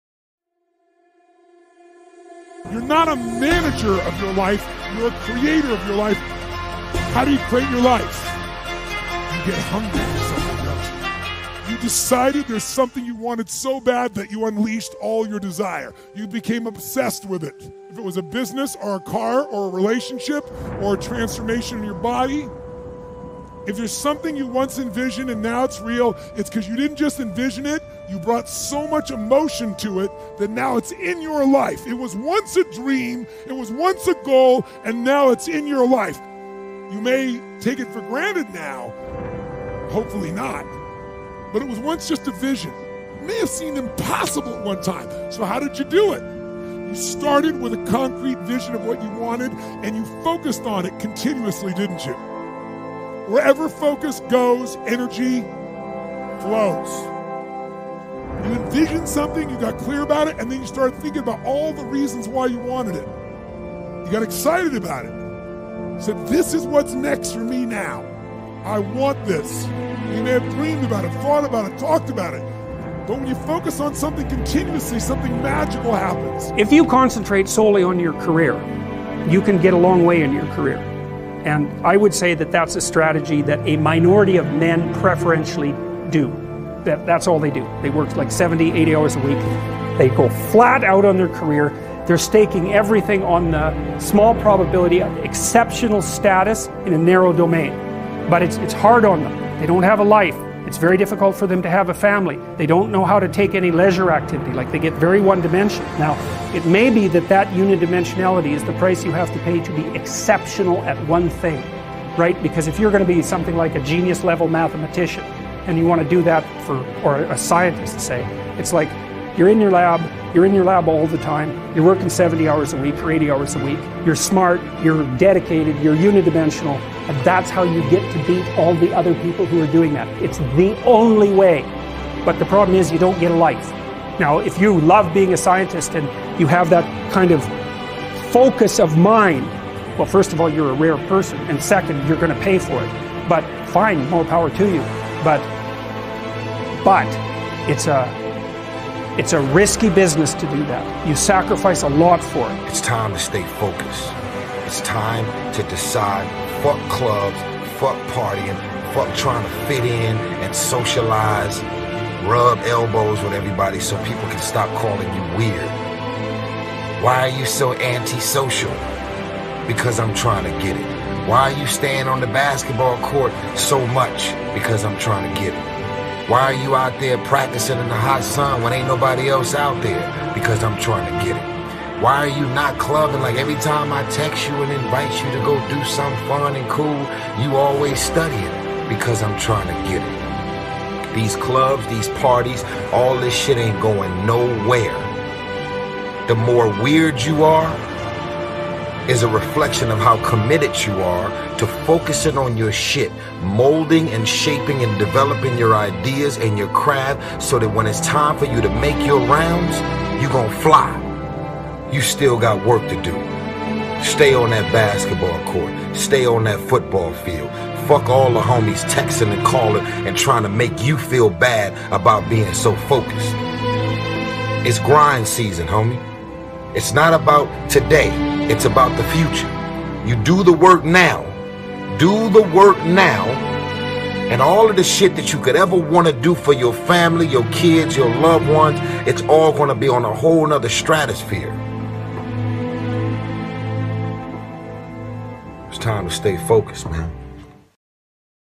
Get HUNGRY and FOCUS on YOURSELF - Best Motivational Speech final
If you want to make some change, now is the time !

Sometime it's important to take a step back and remember what is the most important thing... Just start

▶ SPEAKER:
- Tony Robbins
- Jordan Peterson
- Tyrese Gibson

       ''  Which person changes your way of thinking these days and why?  ”